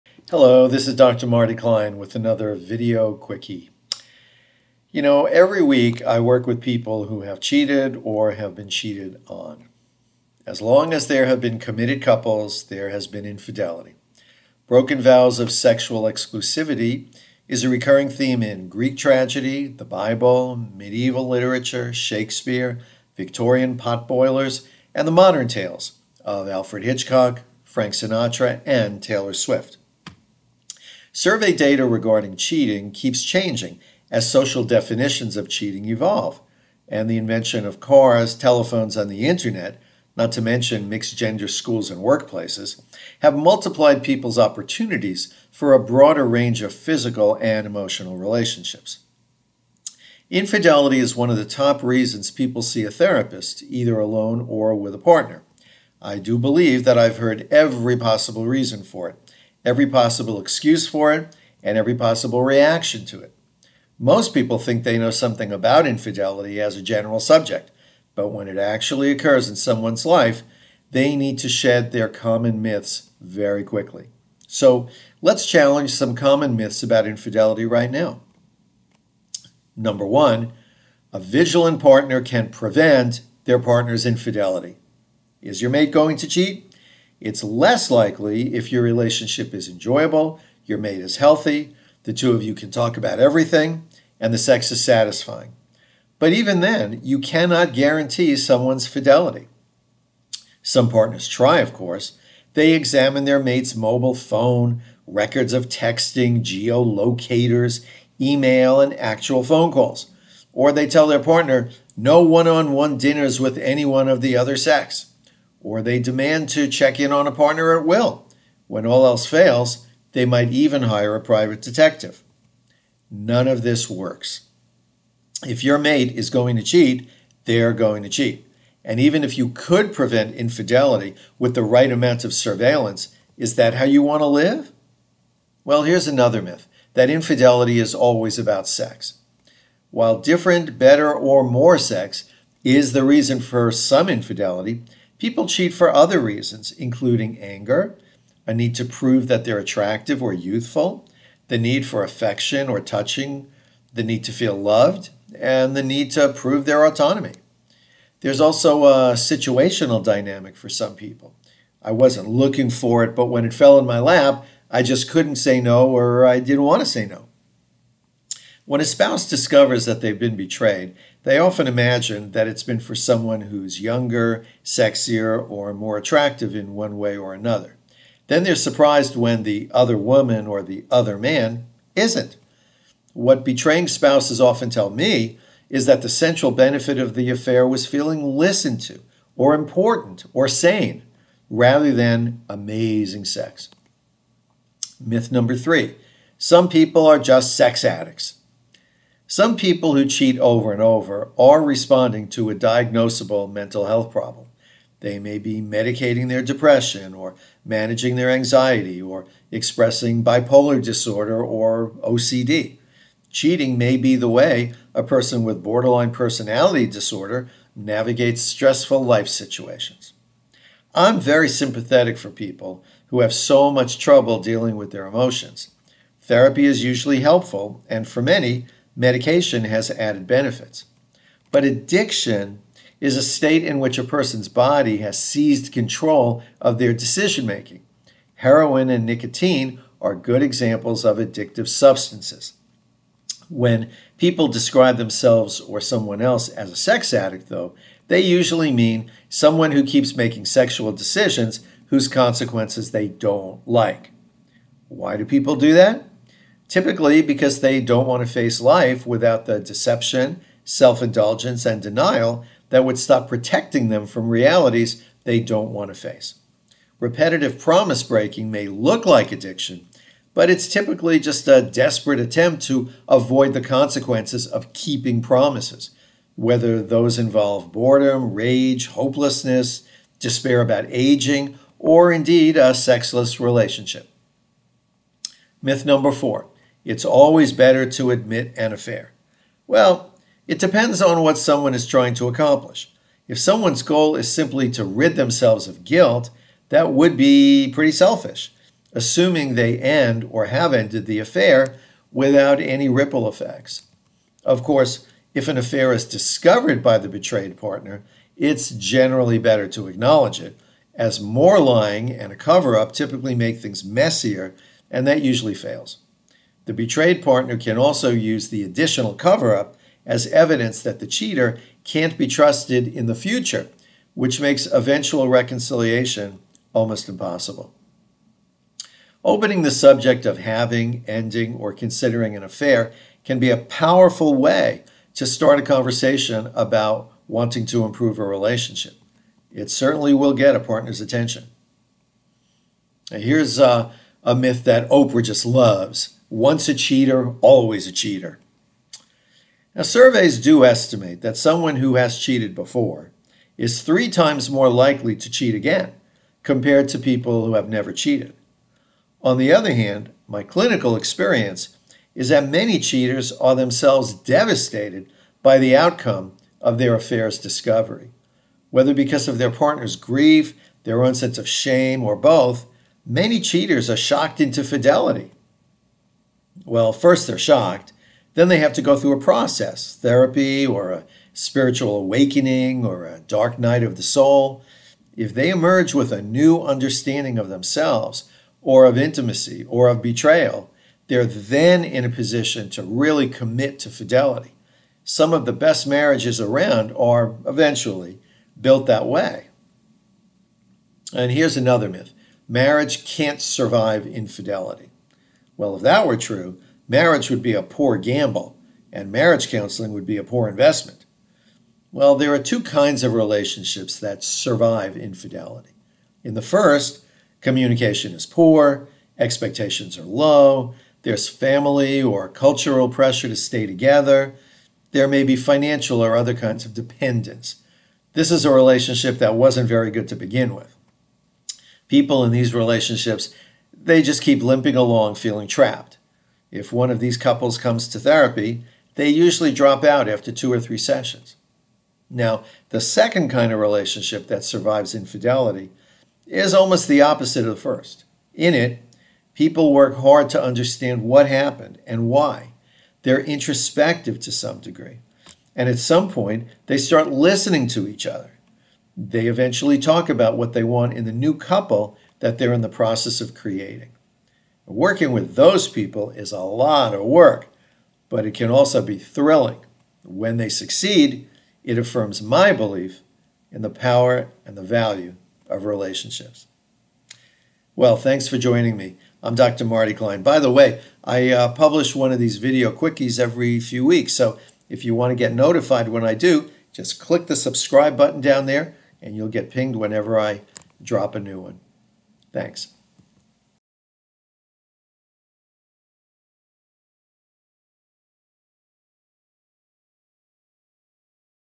Infidelity Myths
As long as there have been committed couples, there has been infidelity--as you can see in the Bible, Shakespeare, and the modern tales of Alfred Hitchcock and Taylor Swift.

Infidelity is one of the top reasons people see a therapist, either alone or as a couple. Most people think they know something about it as a general subject--but when infidelity actually occurs in someone's life, they need to shed common myths about it. And quickly.

Common myths about infidelity include (1) You can prevent your partner from cheating, (2) Affairs are usually about sex; (3) Some people are simply sex addicts; (4) It's always better...